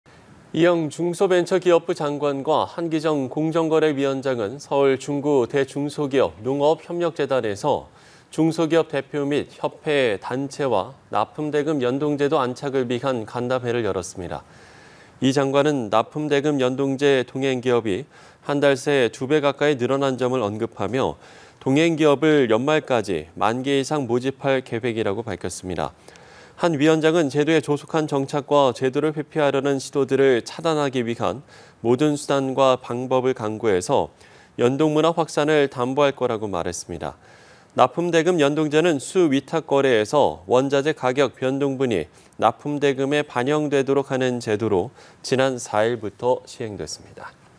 '납품대금 연동제' 참여기업 2배↑…"연말까지 1만개" / 연합뉴스TV (YonhapnewsTV)
'납품대금 연동제' 참여기업 2배↑…"연말까지 1만개"

이영 중소벤처기업부 장관과 한기정 공정거래위원장은 서울 중구 대·중소기업·농어업협력재단에서 중소기업 대표 및 협회·단체와 납품대금 연동제도 안착을 위한 간담회를 열었습니다.

이 장관은 납품대금 연동제 동행기업이 한 달새 2배 가까이 늘어난 점을 언급하며 "동행기업을 연말까지 1만개 이상 모집할 계획"이라고 밝혔습니다.

한 위원장은 "제도의 조속한 정착과 제도를 회피하려는 시도들을 차단하기 위한 모든 수단과 방법을 강구해 연동문화 확산을 담보할 것"이라고 말했습니다.

납품대금 연동제는 수·위탁거래에서 원자재 가격 변동분이 납품대금에 반영되도록 하는 제도로, 지난 4일부터 시행됐습니다.